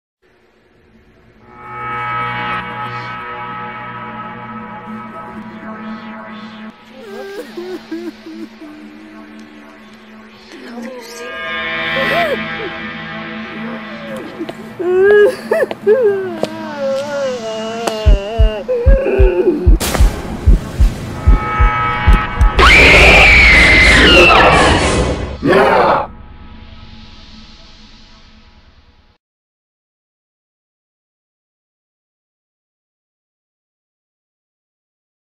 The crying man: My 1st horror short
This is my first short horror film.  Although i'm not making any excuses for myself there's a lot more I could do, like find better sound effects, spend more than 5 minutes filming and maybe using more than just one actor but and this is a HUGE butt I wanted to just dive in with it and just see how it went.  I figured I migh learn a thing or also. And I did.  So much.  If anything this is a really funny short.  And to go with this video i'll upload a video showing you how I did this. Also sorry for the grainy quality.

Only a few hours total filming and editing were spent on this no budget horror short.
All shot with a Samsung galaxy S7